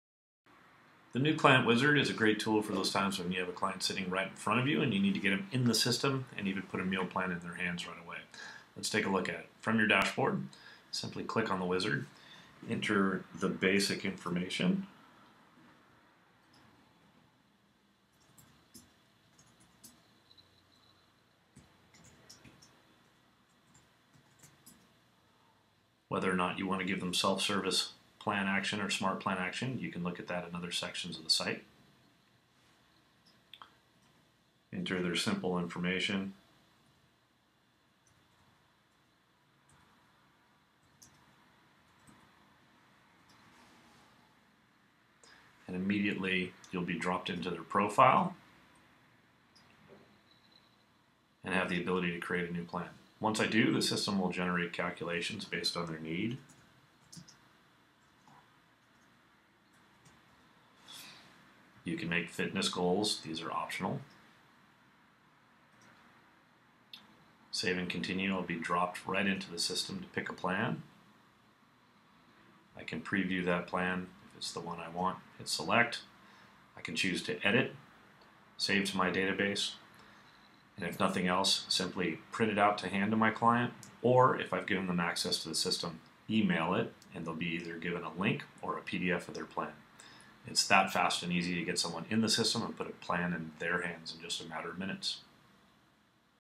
New Client Wizard | Evolution Nutrition
For those times when you have a new client sitting with you and you need to get them in the system, and even hand them their first Pro Plan, the New Client Wizard helps you accomplish that in a matter of minutes, with just the click of a few buttons.